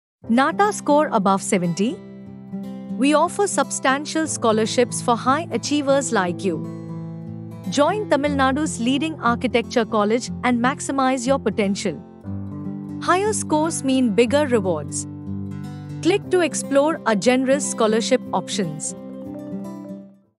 NATA Score Above 70? You're eligible for Scholarships
Click Here to Apply Now!
🎓 Achieved a NATA score of 70 or above? Discover the scholarships available at our prestigious architecture institute. Secure your future in architecture with our Bronze, Silver, and Gold Scholarships! 🌟To learn more and join us today!